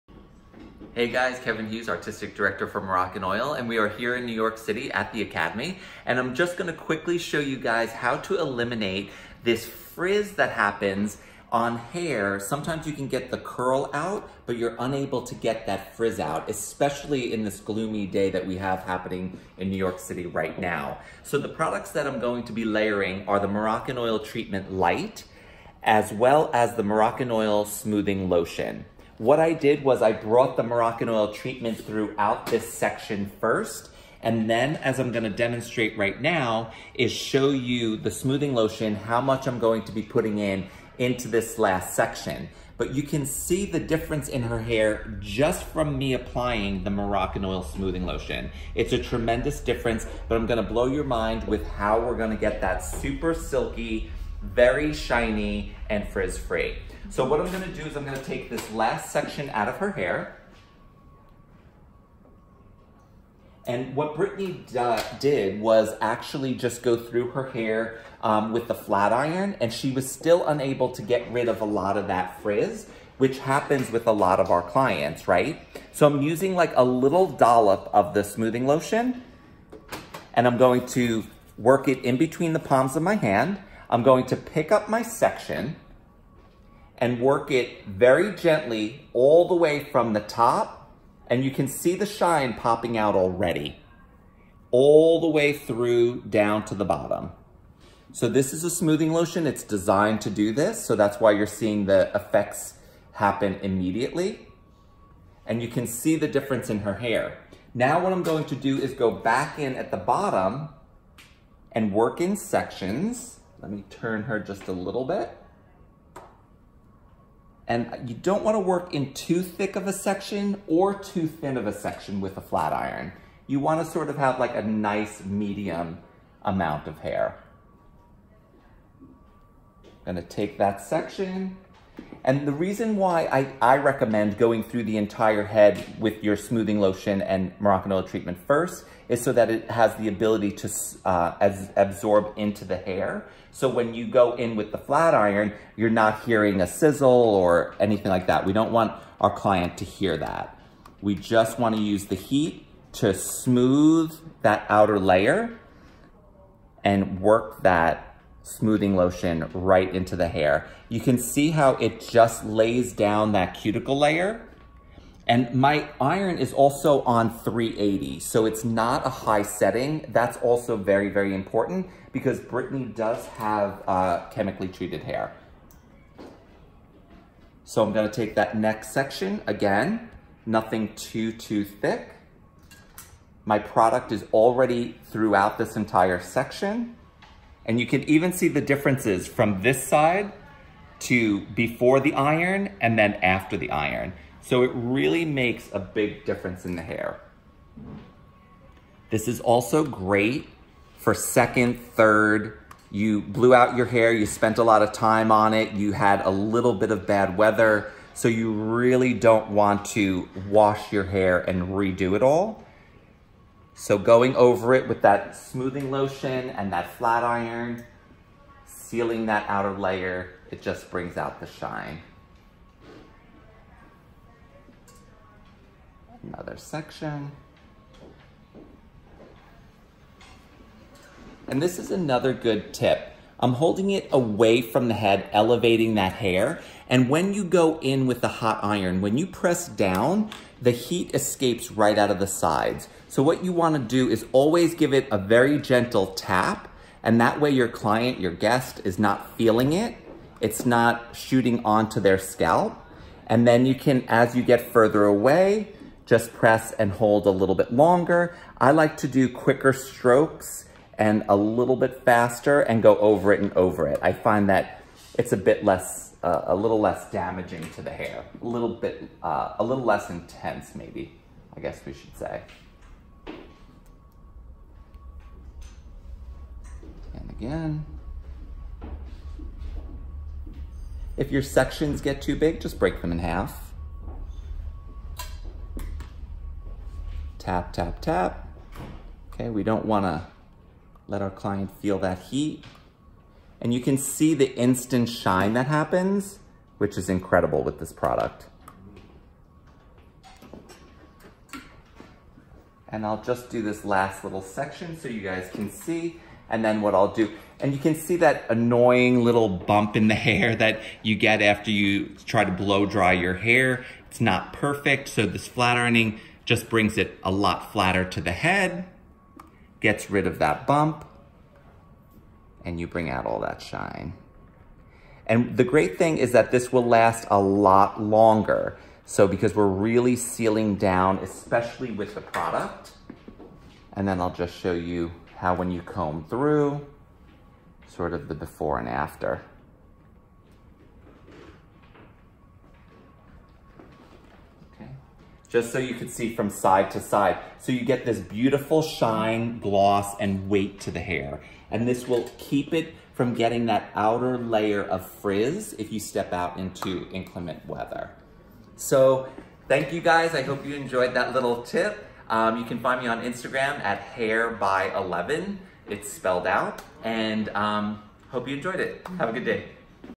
HAIR TUTORIAL: Kevin Hughes Demos How-To Remove Frizz To Get Sleek Hair
Kevin Hughes of Moroccanoil demos how to remove frizz and bulk from the hair.